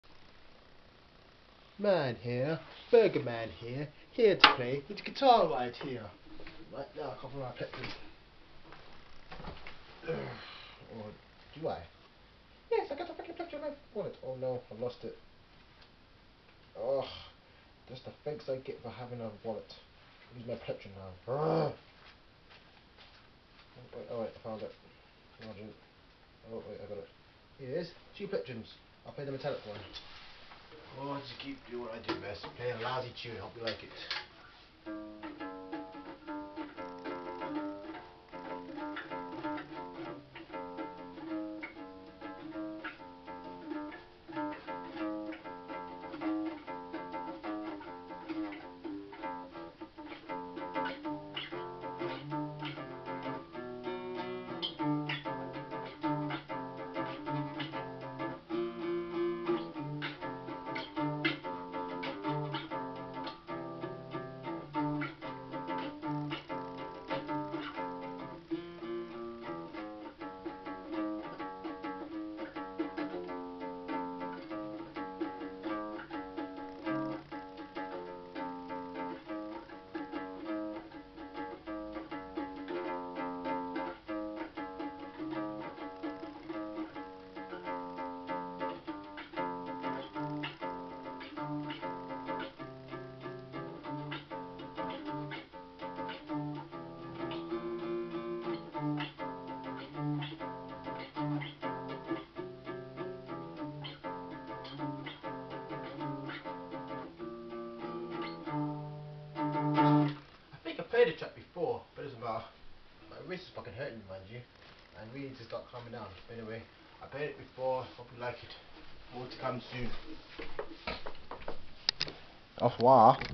BURGER MAN BOB'S GUITAR PLAY CRUD 05 (acoustic)
IF CHEAPTOONS HAD A SCORE ARTIST
THIS IS THE GUY

BURGER MAN BOB ..
NOBODY A BIG DEAL, BUTT HE LOVES TO PLAY HIS GUITAR AND DO NEW TUNES
 CONSIDER HIM AN ABSTRACT GUITAR PLAYER

MORE MAY COME SOON
ALL RIGHTS AND RESERVES ARE FROM BURGERMAN BOB'S MUSIC (C)2020